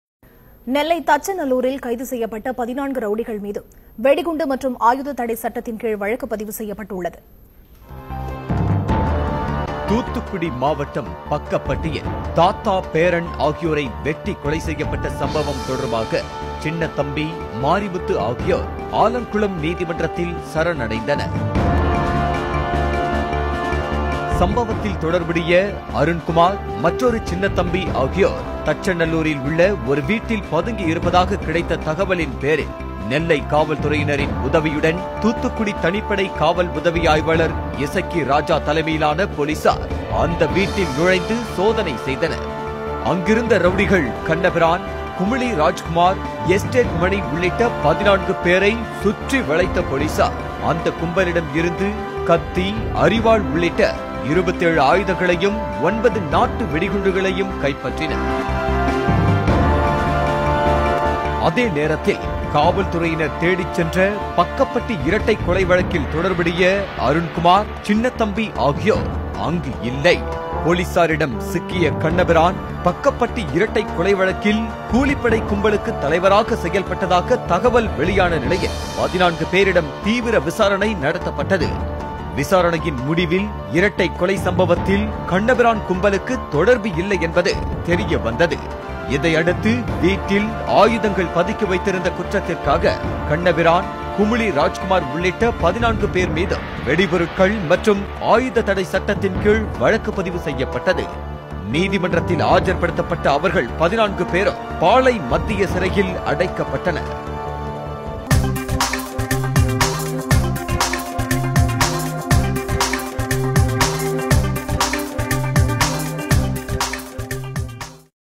Kumlirajkumar | Kannapiran | EstateMani&15 rowdy's With Weapons and bomb Arrested by Si Esakkiraja
Video from M.Marimuthu